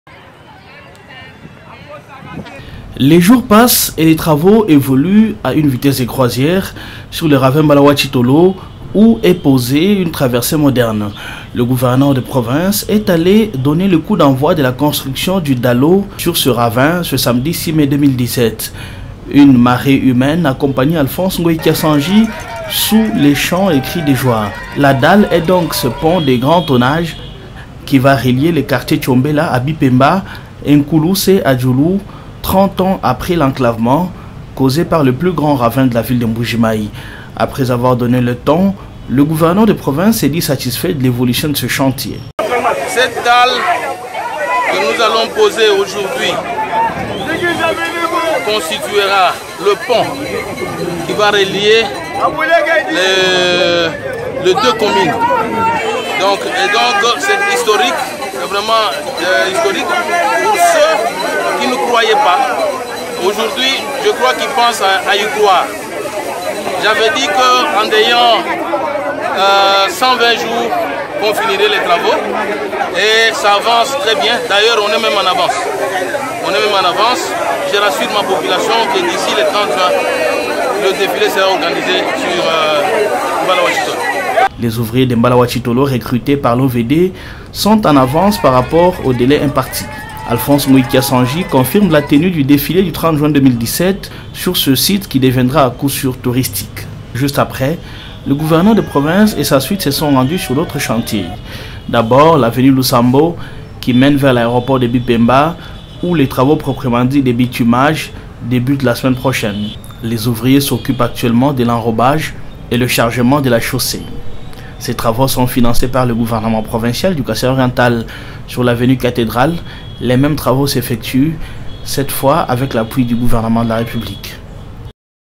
Début de la pose du Dalot sur le ravin Mbala wa Tshitolo à Mbujimayi
Les travaux de la pose du Dalot sur le ravin Mbala wa Tshitolo à Mbuji-Mayi ont débuté ce samedi 06 mai 2017 par la visite du Gouverneur de Province sur ce chantier. Ngoyi Kasanji a lui-même versé le premier pot de ciment à la surface.